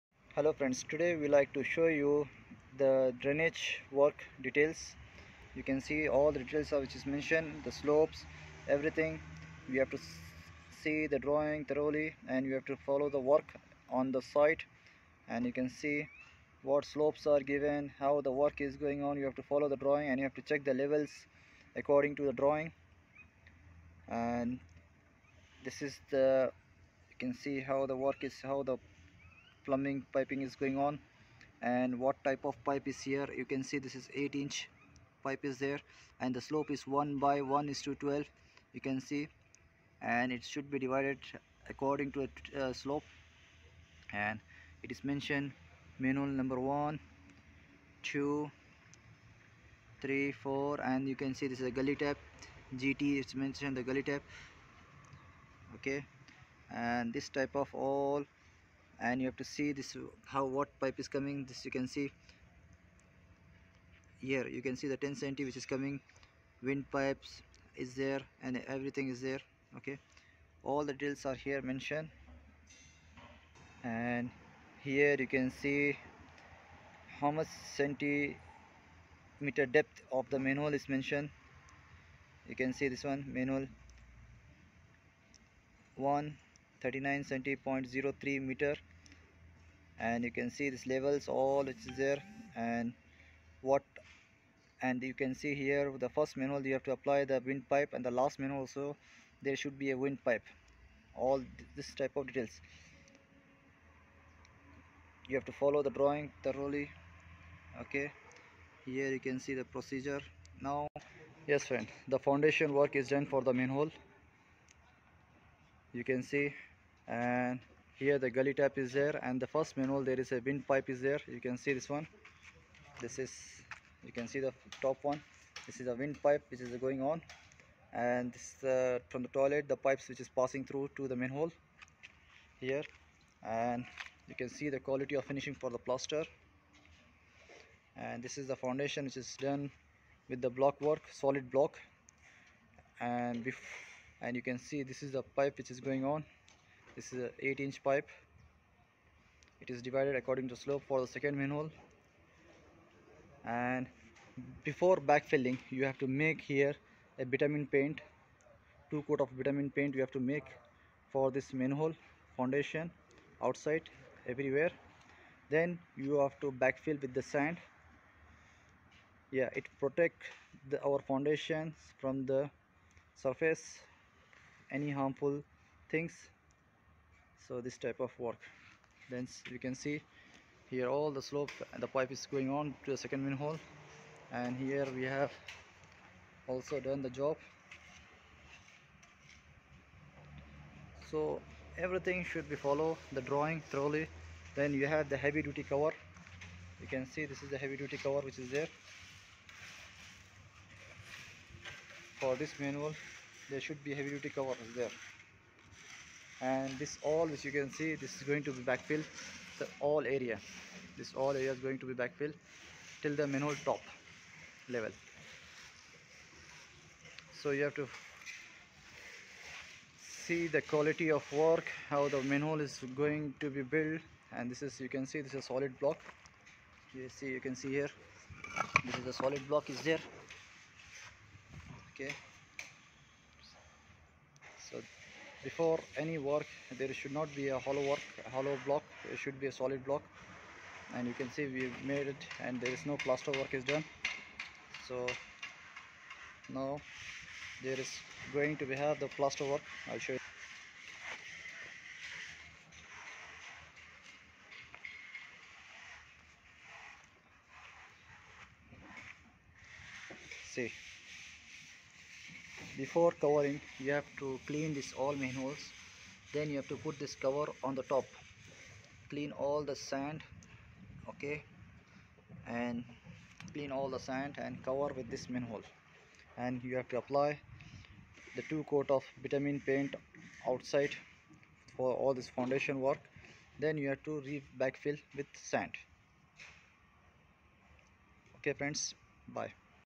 How to do Drainage system works
Your home's drainage system uses gravity to pull water through the house and outside to drain. Waste and dirty water is piped through a “Soil Stack” which in turn takes the waste through a main drain, generally located underneath the house. Vents prevent these gases from escaping through the house. ...